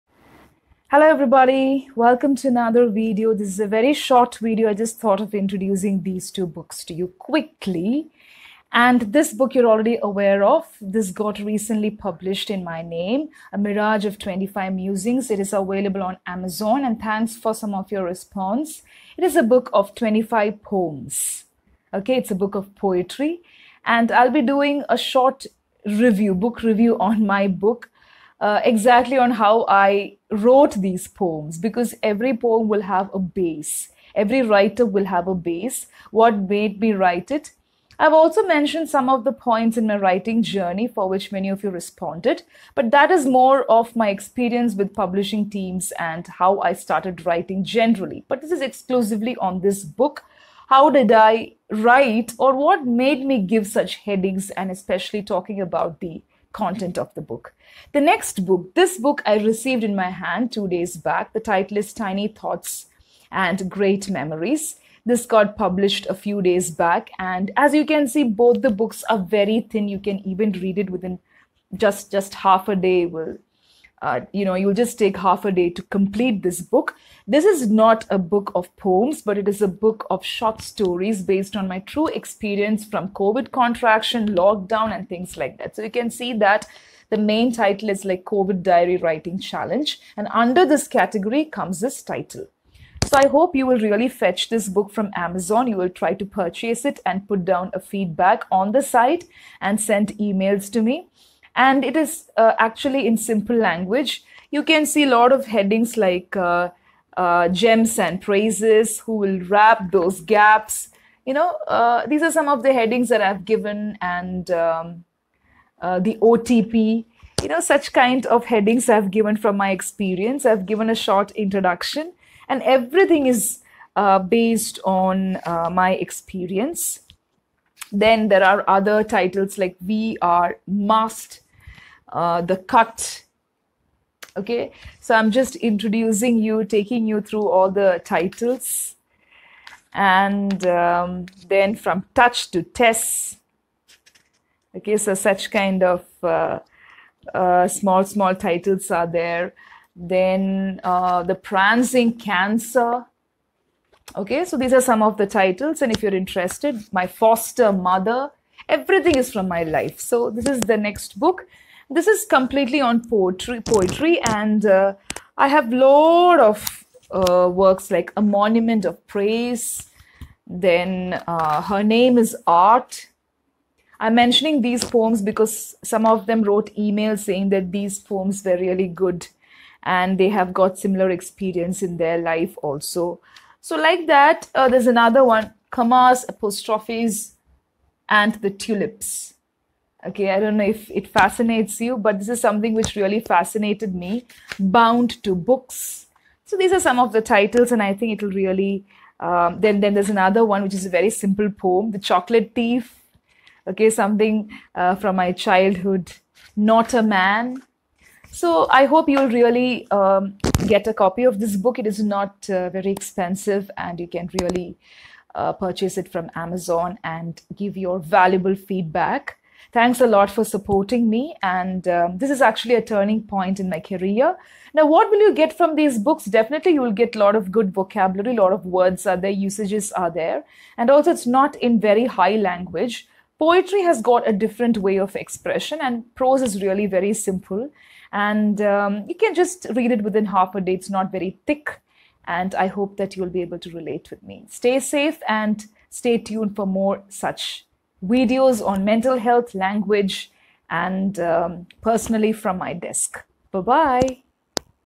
Two in One Surprise
A descriptive video on two of my own books

🌼A MIRAGE OF 25 MUSINGS

🌼 TINY THOUGHTS AND GREAT MEMORIES

Mirage of 25 Musings